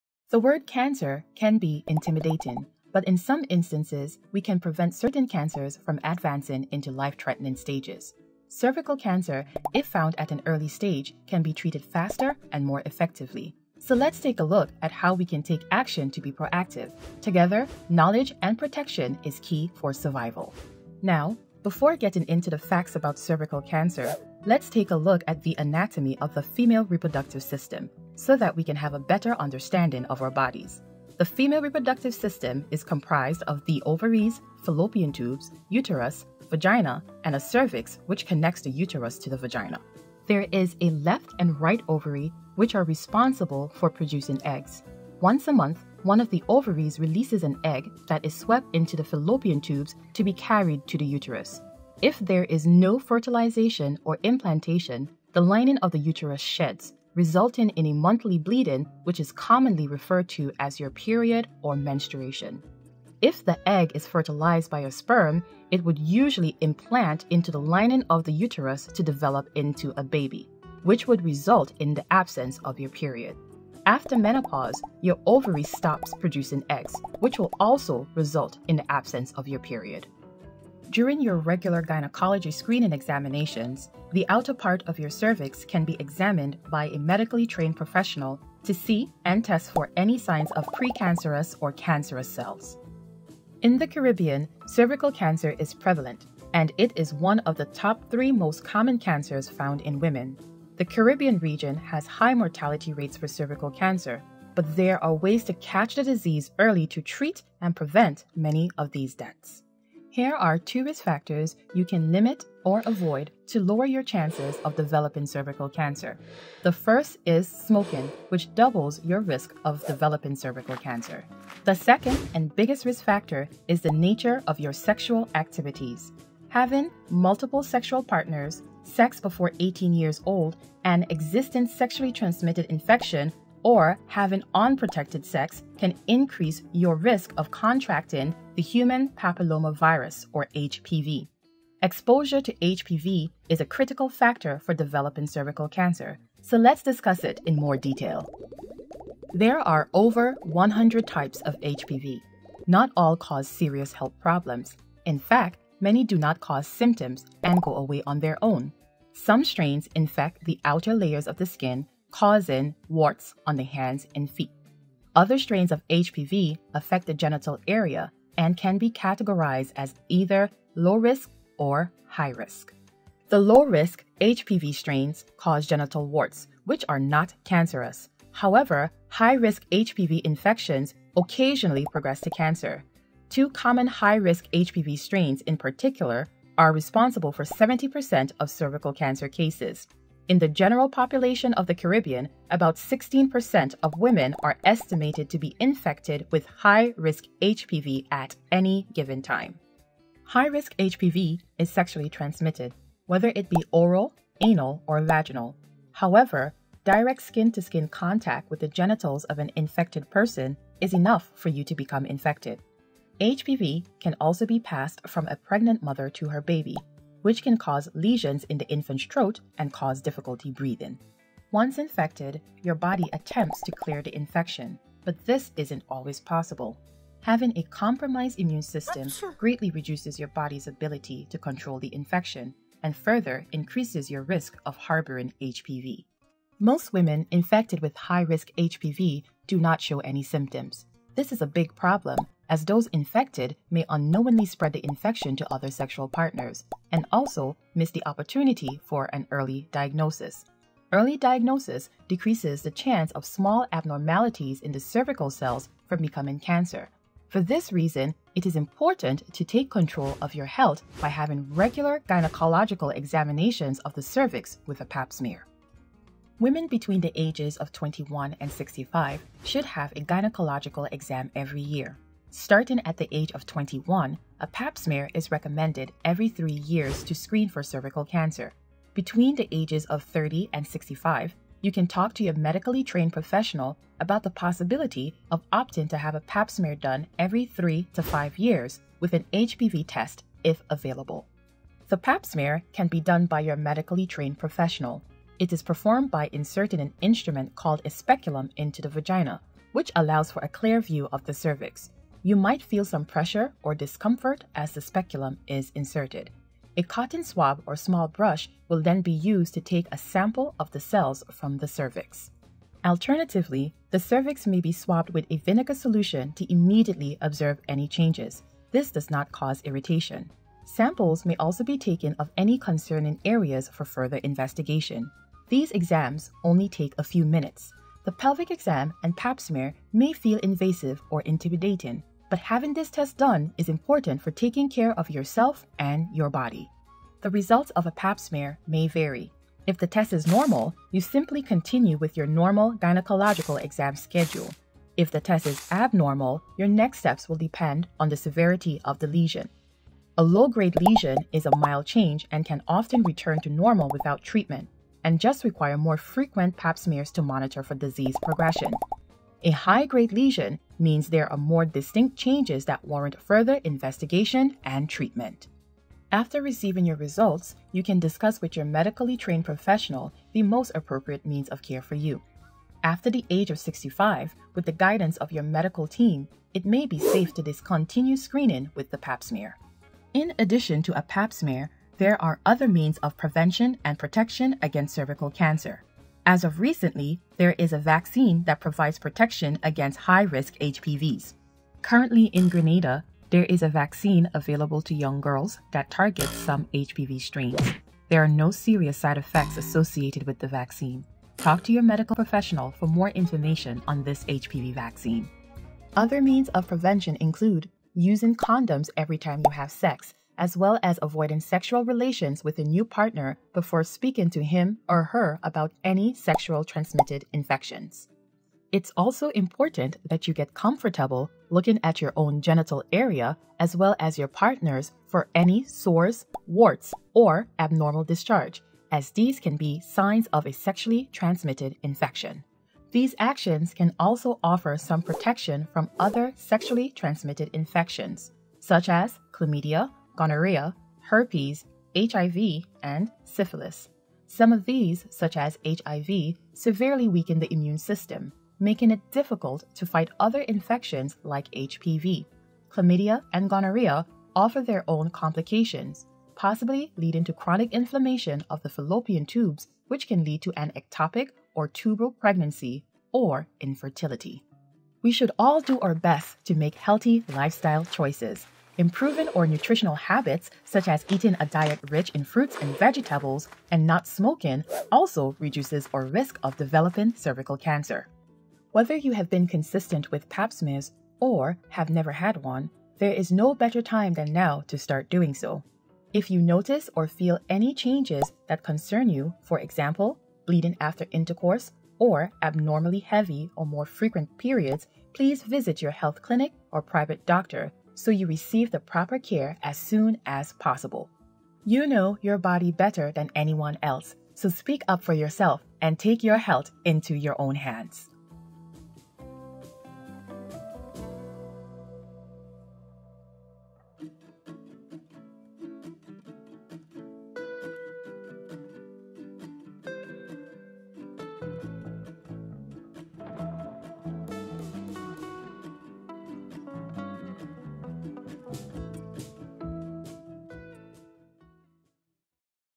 Cervical Cancer - A Public Health Awareness Campaign for the Caribbean Region
This animation is a sample of a larger Public Health Awareness Campaign tailored to the Caribbean Region. The campaign demystifies the causes and effects of the Human Papilloma Virus while encouraging women of all ages to receive regular gynaecological exams to prevent the development of cervical cancer. Cervical cancer ranks among the highest rates globally and is disproportionately affecting women in low-income communities. Grenada itself carries a high cervical disease burden and associated mortality rate. In collaboration with health experts of Caribbean origin, we co-wrote a script which includes regional language cues and lay-audience vocabulary. We illustrated assets to reflect local characteristics and staged animations with subtle humour to resonate with the Grenadian audience using personally recognisable situations. The campaign focuses on best practices for prevention and aims to fill the need for more culturally appropriate health education materials in the Caribbean region.

In collaboration with Women in Medicine.

@2021, St. George's University